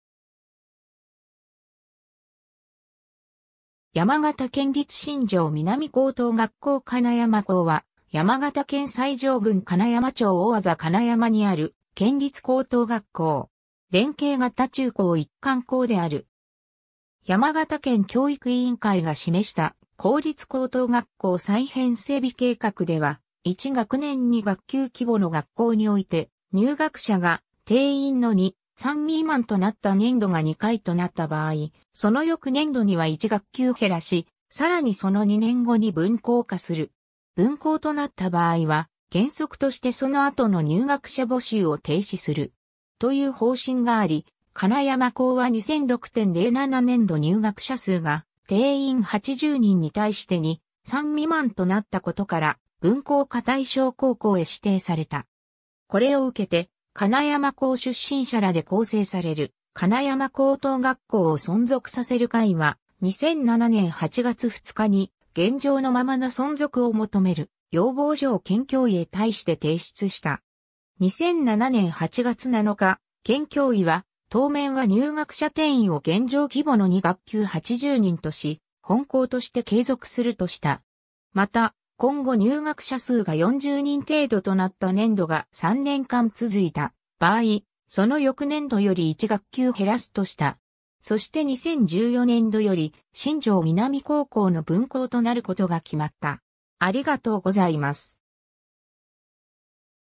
山形県立新庄南高等学校金山校
金山町_(山形県)
山形県立新庄南高等学校金山校（やまがたけんりつ しんじょうみなみこうとうがっこうかねやまこう、Yamagata Prefectural Shinjo Minami High School Kaneyama Branch School）は、山形県最上郡金山町大字金山にある県立高等学校。
連携型中高一貫校である。
 山形県教育委員会が示した公立高等学校再編整備計画では、「1学年2学級規模の学校において、入学者が定員の2/3未満となった年度が2回となった場合、その翌年度には1学級減らし、更にその2年後に分校化する。分校となった場合は、原則としてその後の入学者募集を停止する。」という方針があり、金山高（当時）は2006・07年度入学者数が、定員80人に対して2/3未満となったことから、分校化対象高校へ指定された。
これを受けて金山高出身者らで構成される金山高等学校を存続させる会は、2007年8月2日に「現状のままの存続」を求める要望書を県教委へ対して提出した。
2007年8月7日、県教委は、当面は入学者定員を現状規模の2学級80人とし本校として継続するとした。
また、今後入学者数が40人程度となった年度が3年間続いた場合、その翌年度より1学級減らすとした。
 そして2014年度より新庄南高校の分校となることが決まった。